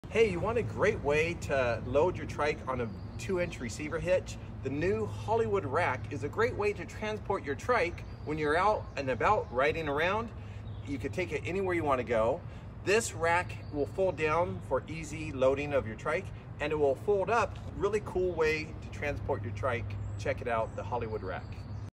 Car Rack to Transport a Heavy E-Trike!
CAR RACK HOLLYWOOD ETRIKE KIT HR1500 with Adapter kit TO FIT ONE ETRIKE